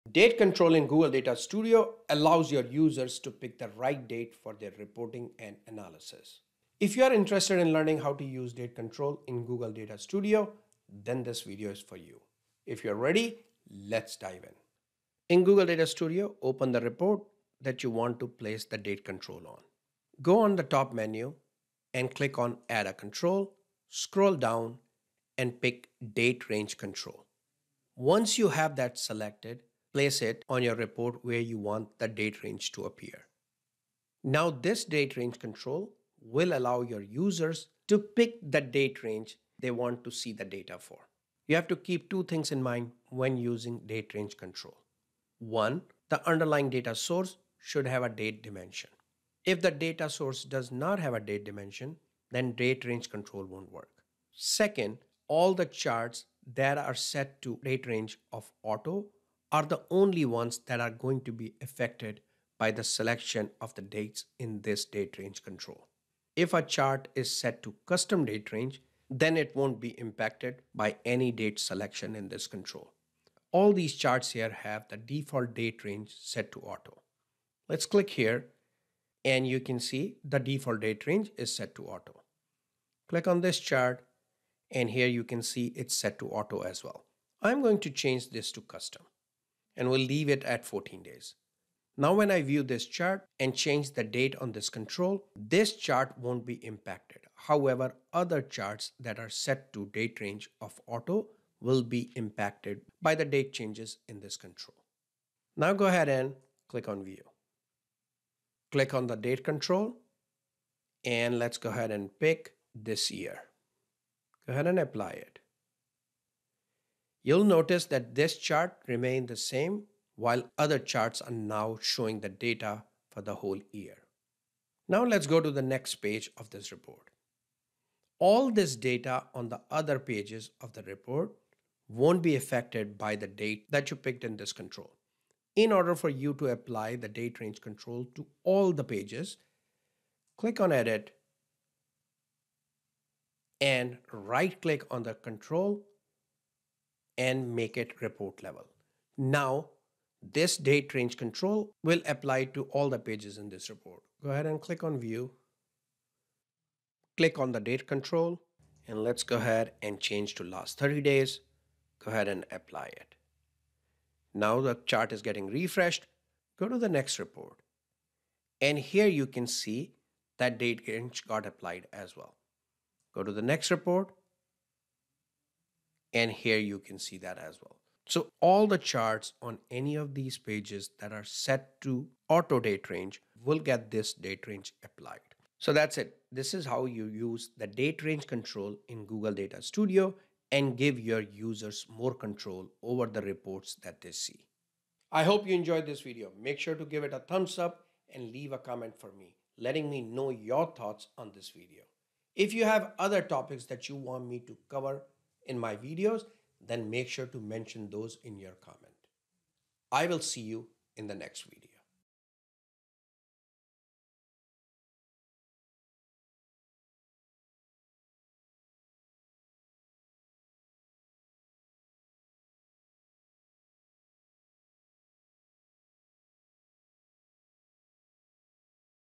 Date Range Control in Google Data Studio - Learn Data Studio Step-by-Step - Beginners Tutorial
Date Range Control Data Studio - Learn Data Studio Step-by-Step - Beginners Tutorial.

In this beginners tutorial learn two important things that will determine if the date range control will work or not.

👉 Free Google Data Studio Training Resources for you

Video Chapters

00:00 Introduction
00:13 Google Data Studio report
00:22 Finding the Date Range Control in Google Data Studio
00:33 Placing the date range control
01:14 Making sure control works and setting a custom date range
01:57 Viewing the report with Date Range control
02:23 Applying the date range control to all the pages in the Google Data Studio report